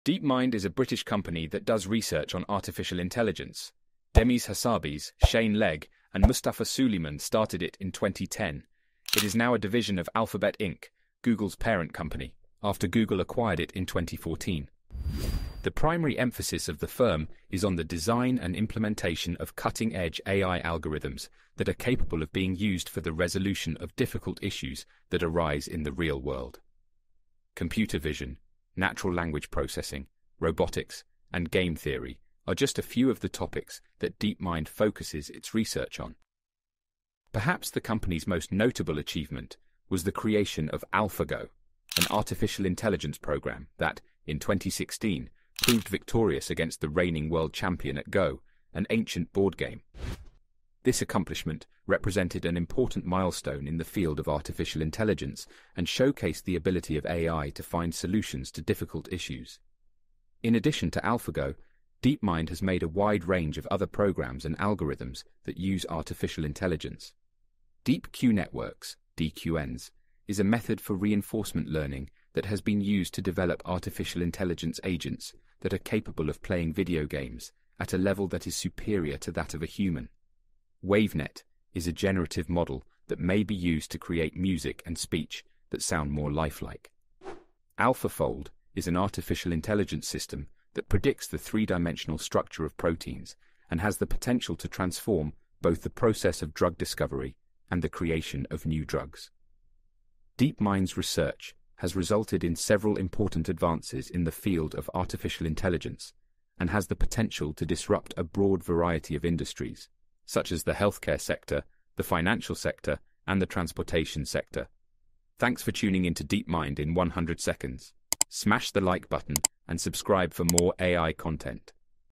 Deepmind explained in 2 minutes
Dive into the groundbreaking world of DeepMind in just two minutes! This concise yet detailed video takes you on a whirlwind tour of DeepMind, the renowned artificial intelligence research lab. Discover how DeepMind's trailblazing advancements in machine learning and AI are reshaping our understanding of technology and its potential. From beating human champions in complex games like Go and Chess to tackling real-world problems, DeepMind stands at the forefront of AI innovation. This video serves as an excellent primer for anyone intrigued by AI, machine learning, and the future of technology.